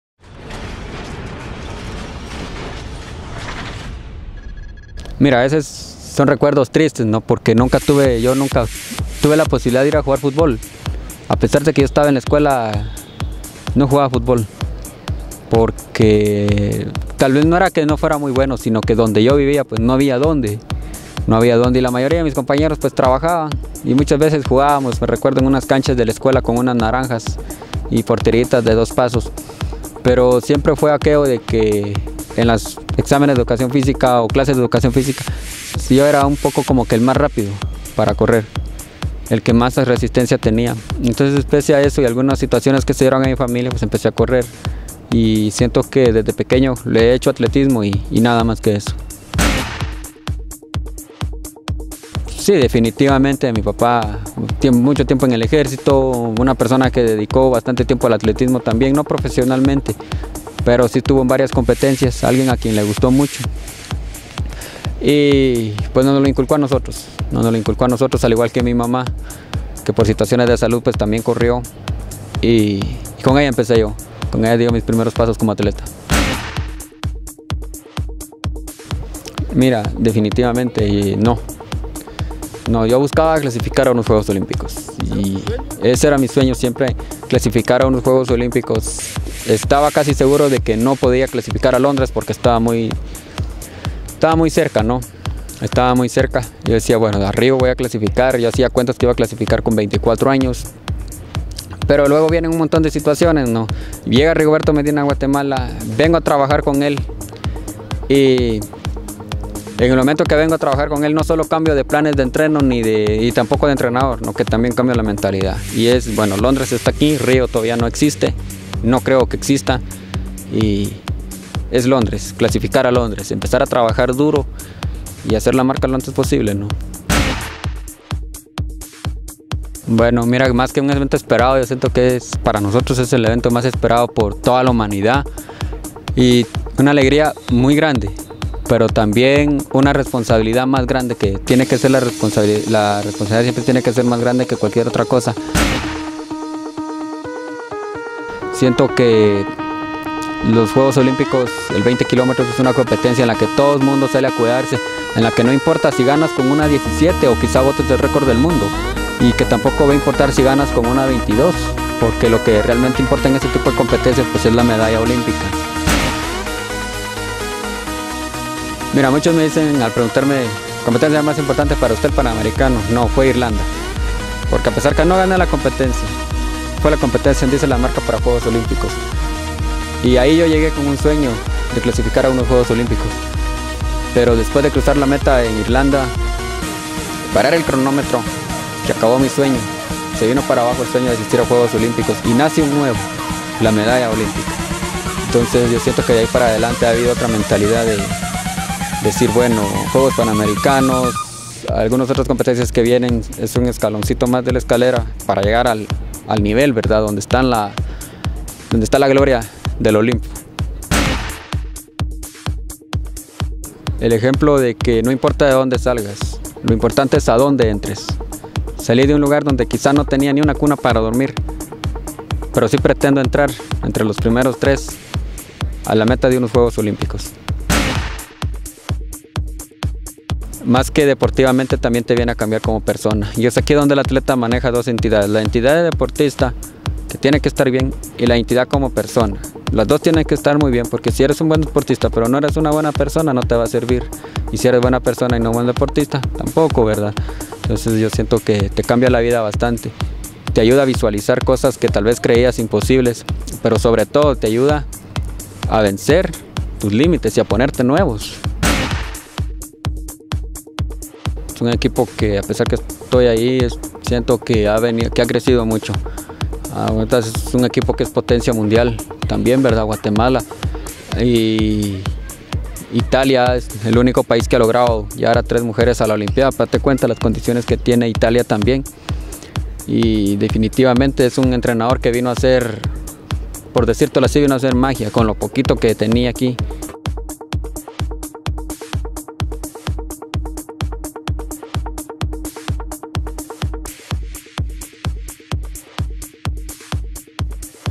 Entrevista a Erick Barrondo, marcha 20 y 50 kilómetros
Entrevista al marchista Erick Barrondo antes de regarlarle a Guatemala la primer medalla Olímpica, Gracias Erick Barrondo por tu humildad, sencillez y espirito GANADOR